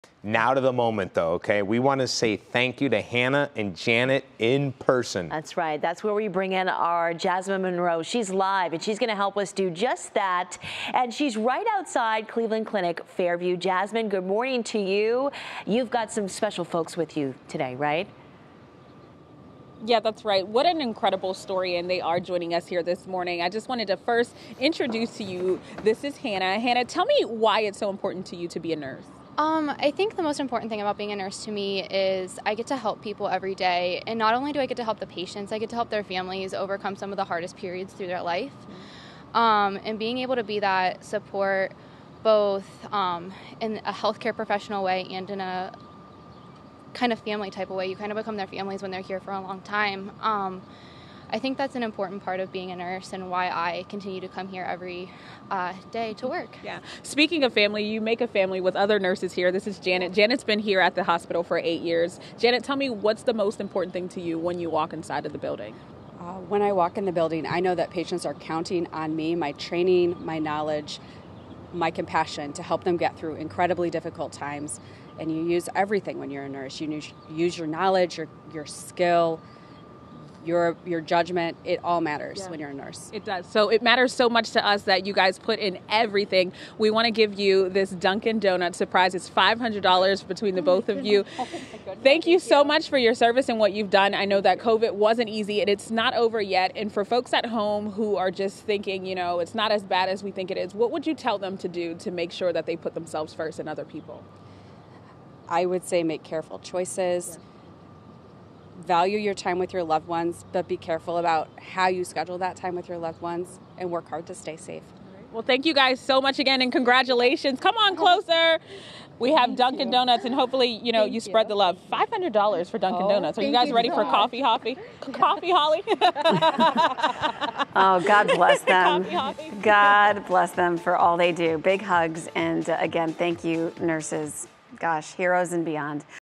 A special surprise for 2 nurses at Cleveland Clinic Fairview Hospital
As we celebrate National Nurses Week, we have a special surprise for two nurses at the Cleveland Clinic Fairview Hospital in honor of all their hard work.

We featured these nurses in a previous story for their efforts in helping save a woman from Berea amid her battle with COVID.